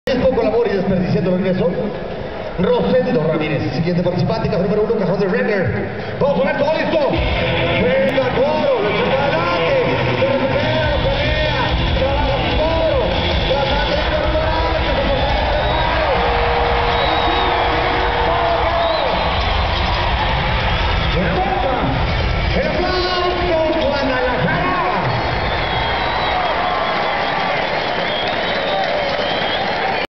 Rosendo Ramirez en Cuernos Chuecos Guadalajara 2009
Rosendo Ramirez en Tour 2009 Cuernos Chuecos
Plaza de Toros Nuevo Progreso
Guadalajara, Jalisco
28 abril 2009